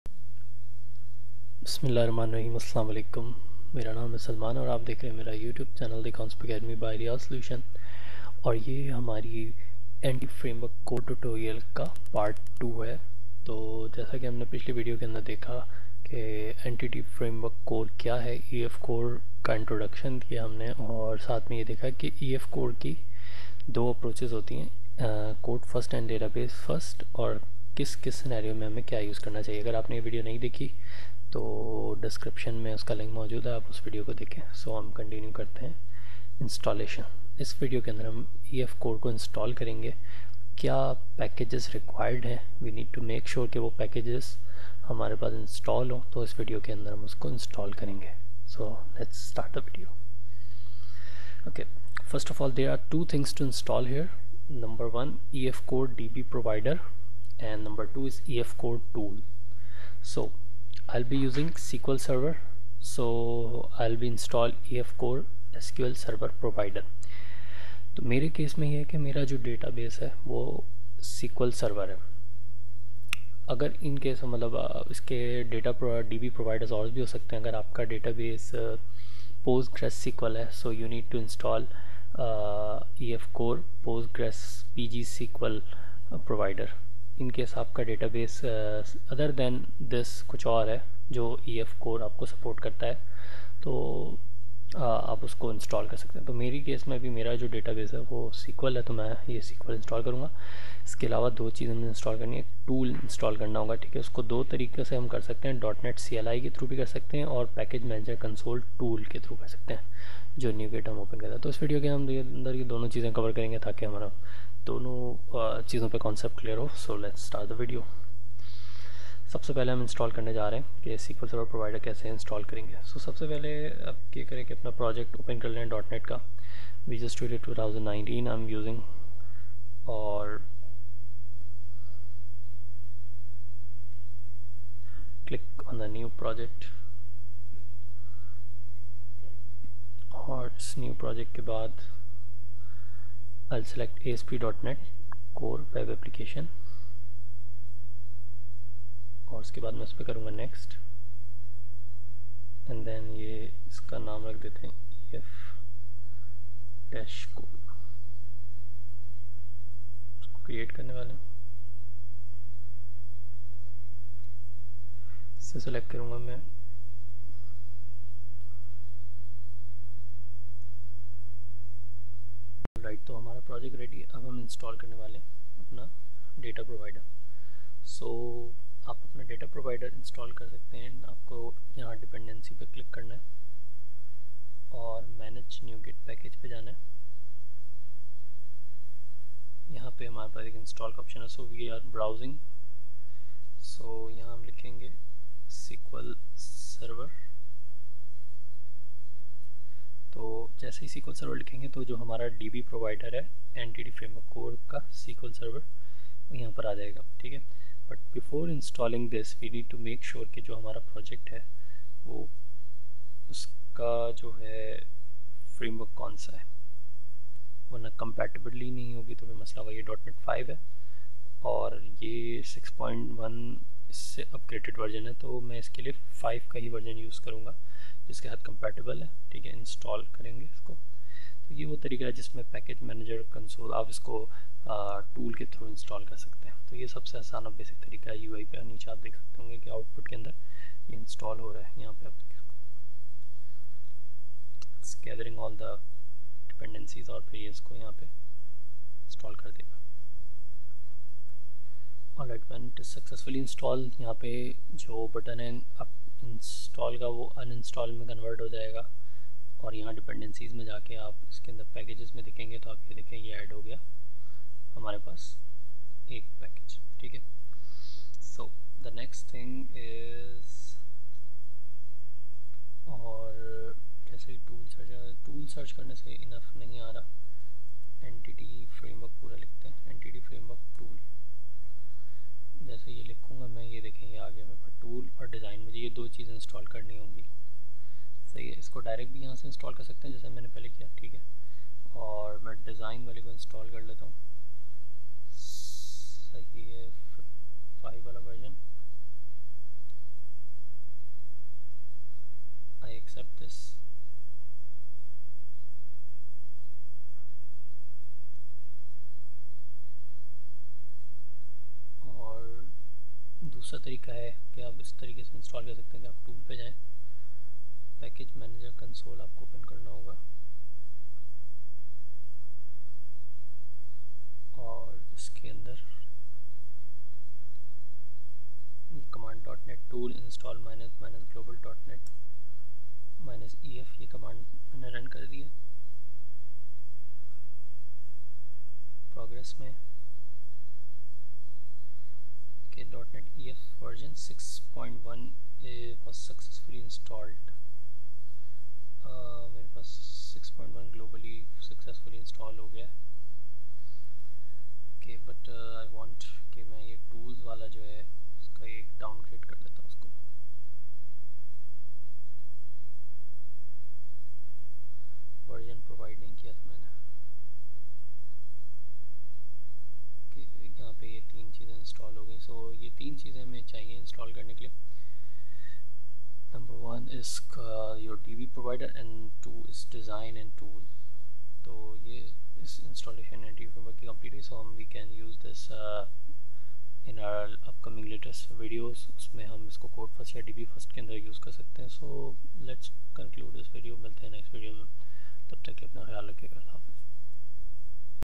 Install Entity Framework Core in asp.net core |How to use EF Core | EF Core in Urdu & Hindi | Part-2
Install EF Core using NuGet Package
There are 2 things to install here:
EF Core DB provider – I will be using SQL Server so I will install EF Core SQL Server provider.
EF Core tools – These are of 2 types – .NET Core command-line interface (CLI) tools & Package Manager Console (PMC) tools.
Install EF Core SQL Server Provider

EF Core has DB providers of all major databases. The installation of each of them is done through NuGet. Here I will tell you how to install a DB provider for SQL Server database.

Install EF Core Tools
First open Package Manager Console window (Tools ➤ NuGet Package Manager ➤ Package Manager Console) then install dotnet ef by running the following command.
dotnet tool install --global dotnet-ef